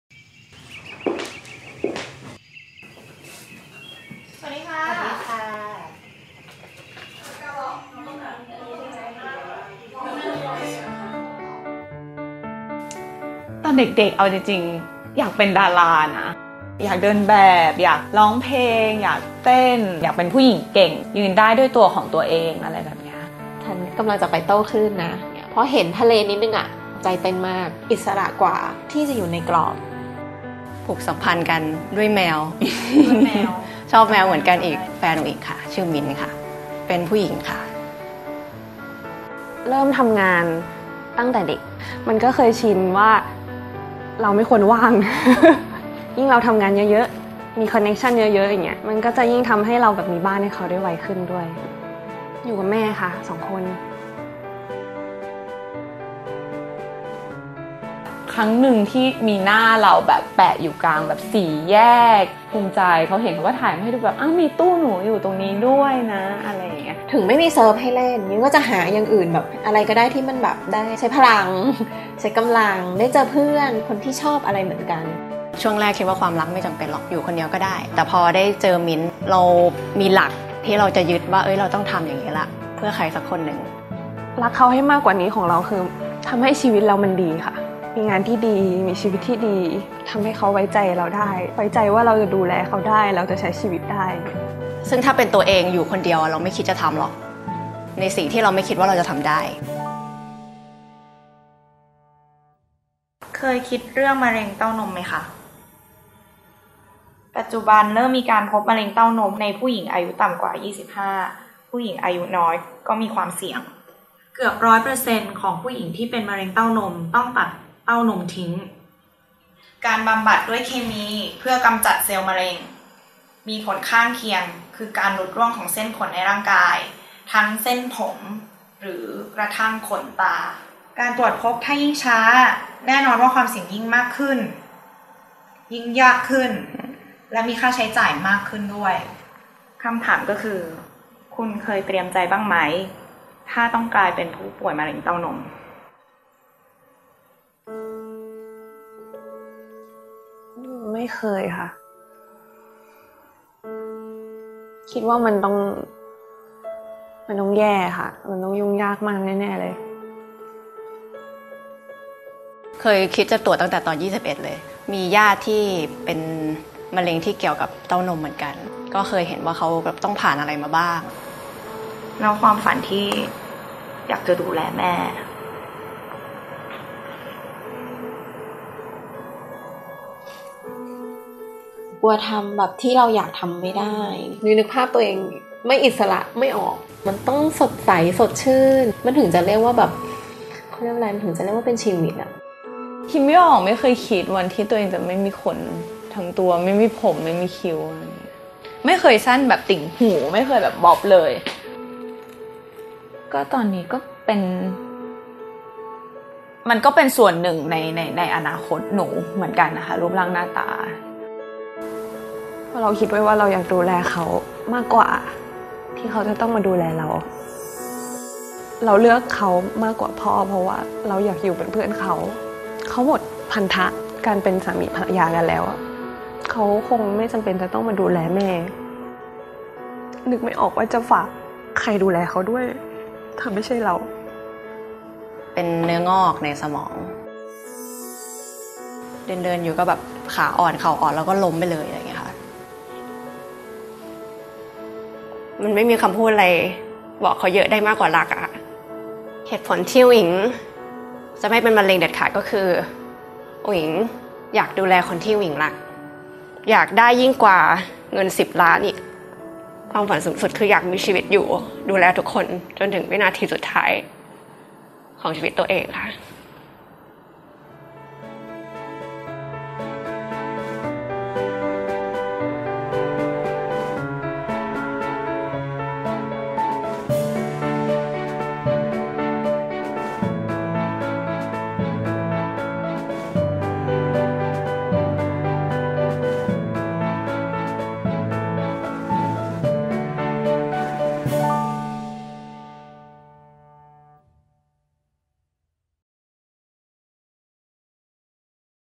OCTOBER STOP CANCER เราจะไม่ปล่อยให้มะเร็ง ทำลายความฝันของผู้หญิง
โรคมะเร็งเต้านม สาวๆ หลายๆ คนอาจจะคิดว่าเป็นเรื่องไกลตัว แต่ความจริงมันใกล้ตัวกว่าที่คิดนะ ยิ่งถ้าเรามีความฝัน ยังอยากทำอะไรมากมาย แต่ทุกอย่างต้องมาหยุดเพียงเพราะเราไม่ใส่ใจตัวเองมากพอ เราว่ามันไม่คุ้มเลย! สาวๆ ที่ไม่อยากให้ความฝันต้องสะดุด รีบตรวจคัดกรองมะเร็งกันได้ตลอดเดือนตุลาคมนี้ ที่โรงพยาบาลพญาไท เพราะเขา “จะไม่ปล่อยให้มะเร็งทำลายความฝันของผู้หญิง”OctoberStopCancer